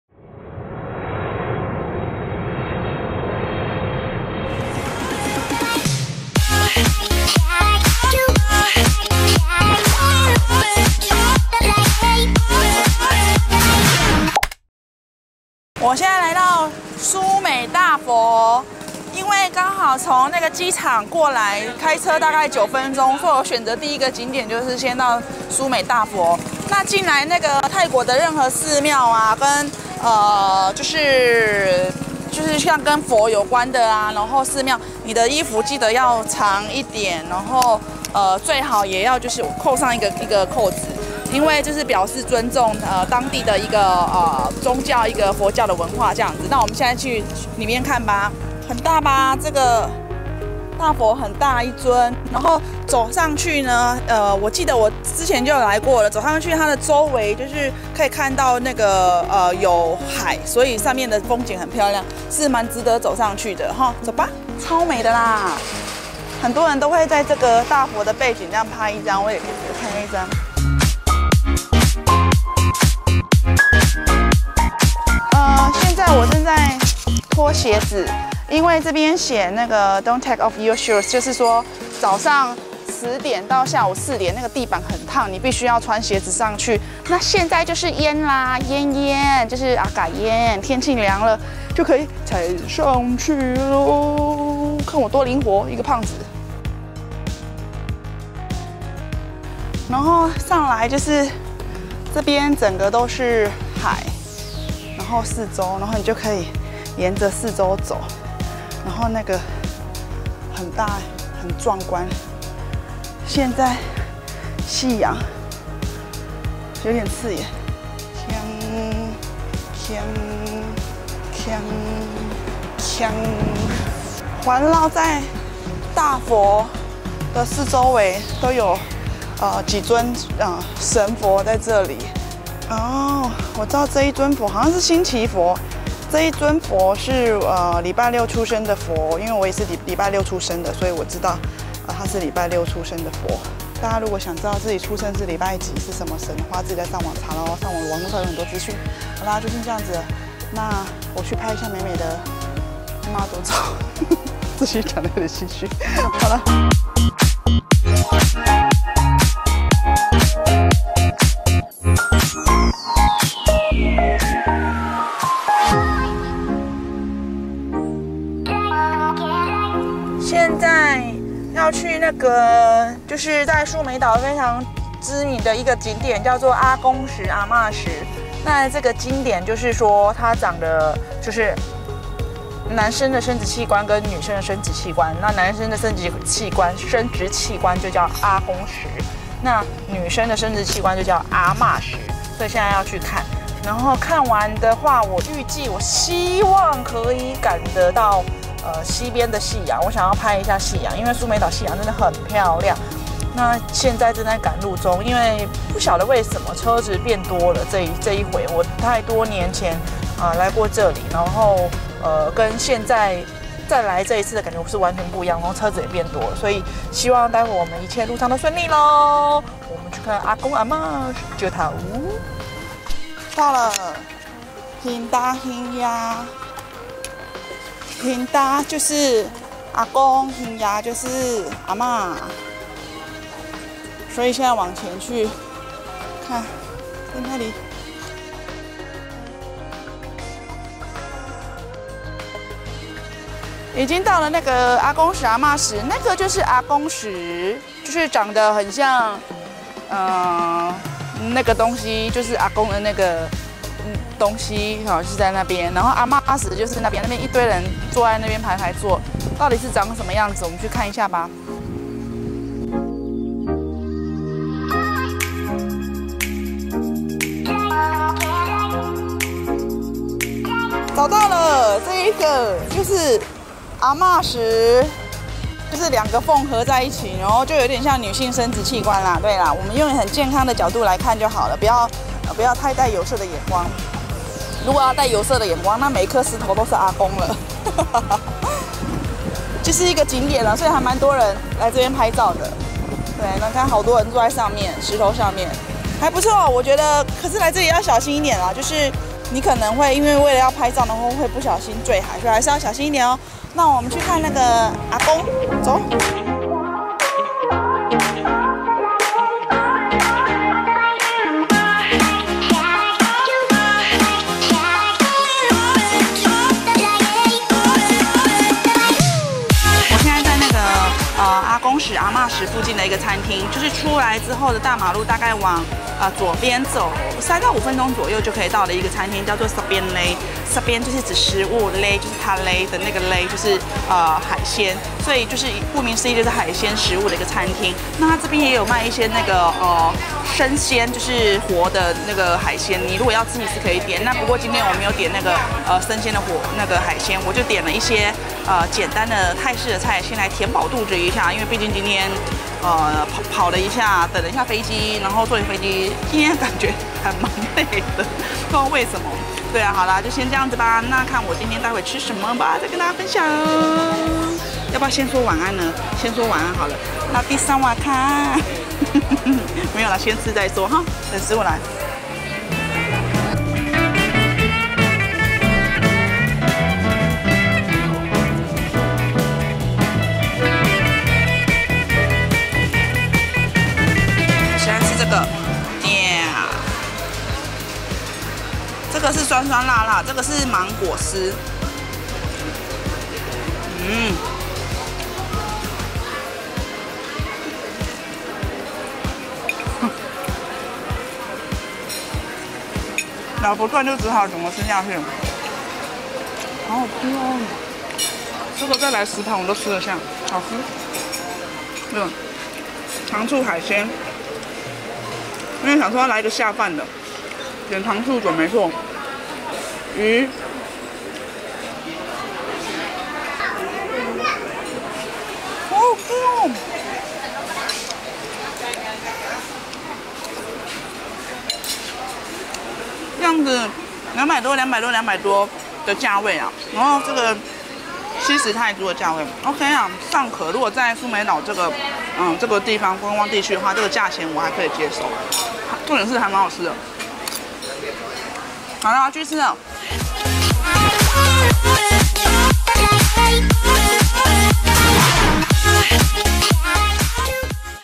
剛下飛機第一天到蘇梅島能去哪？泰國渡假勝地蘇梅島｜蘇梅島景點｜蘇梅島大佛｜蘇梅島阿公石阿嬤石
蘇梅島渡假萬萬歲！

第一天我大概下午到達蘇梅島，節省時間先不去辦入住酒店手續，跟著我一下飛機就開始跑景點行程吧！（行程懶人勿看，你光看就累XD）

第二天一早開始跑透透，恨不得自己的血全都是能量飲料做成的，這樣就不會覺得累了，哈哈哈，跑了一整天，去好多地方也覺得值得啦！

我的影片行程都是給大家參考的，你不一定要照我這樣跑，不過若照我這樣跑的話，我只能說你可以拍很多照片帶走更多美麗的風景！（分幾天跑也是可以啦）